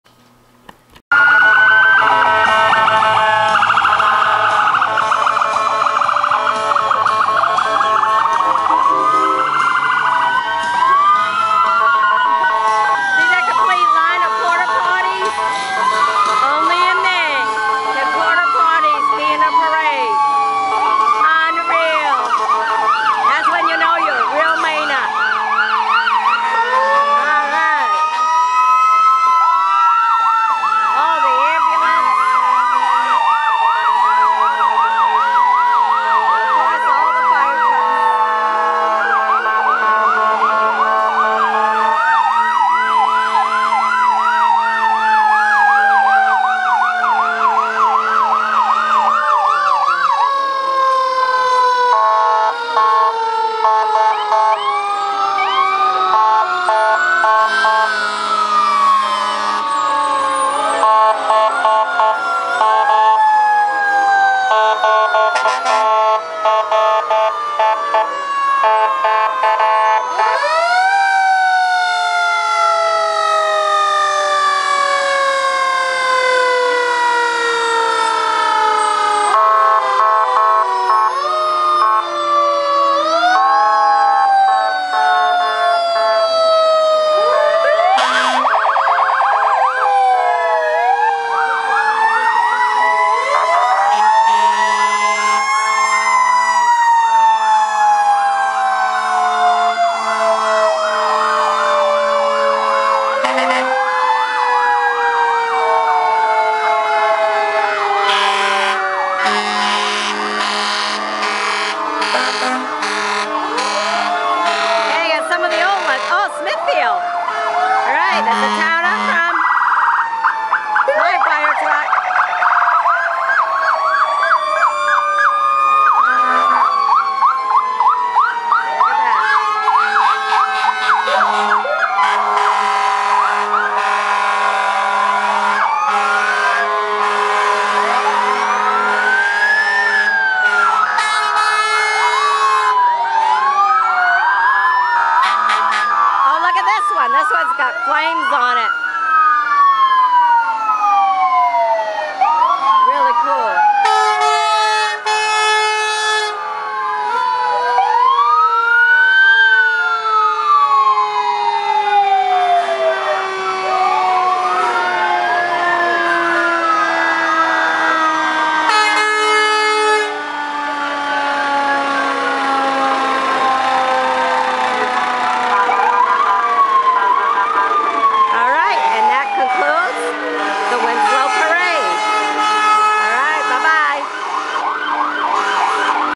Sceaming Fire Trucks
From all the towns around.If you enjoyed this video please leave a comment on my google+ page.